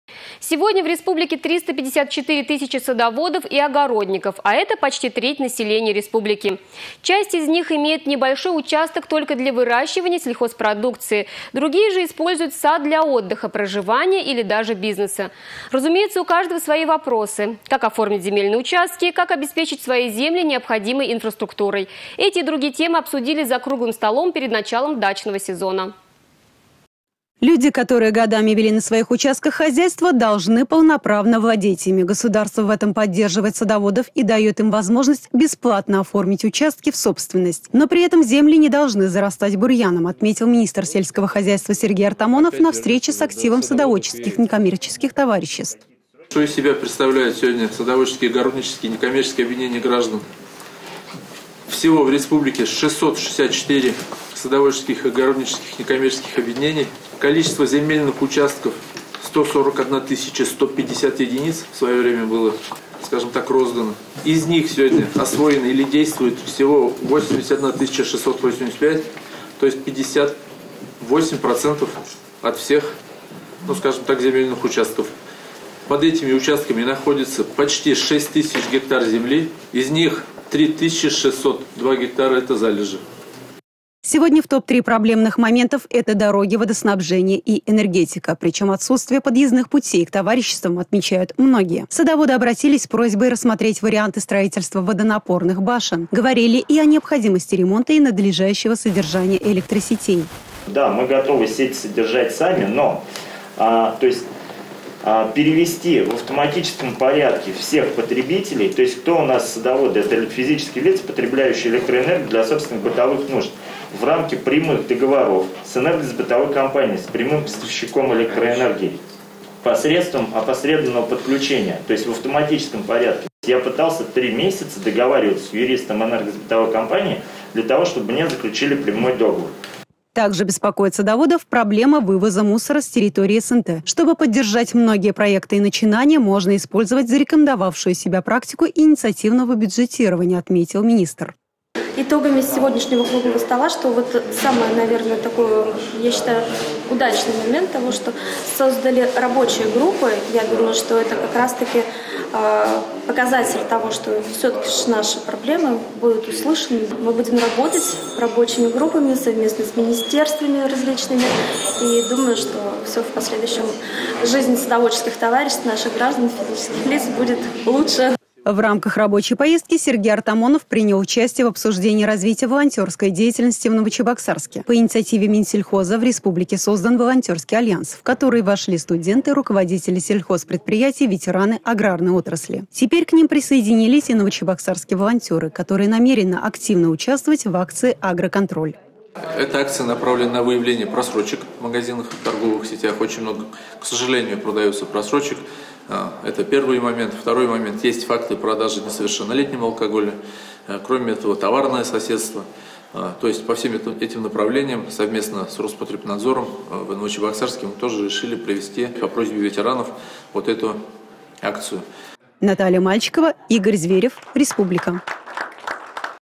Правительство республики окажет садоводам помощь по всем направлениям
Сегодня в республике 354 тысячи садоводов и огородников, а это почти треть населения республики.  Часть из них имеет небольшой участок только для выращивания сельхозпродукции, другие же используют сад для отдыха, проживания или даже бизнеса. Разумеется, у каждого свои вопросы.  Как оформить земельные участки? Как обеспечить свои земли необходимой инфраструктурой? Эти и другие темы обсудили за круглым столом перед началом дачного сезона.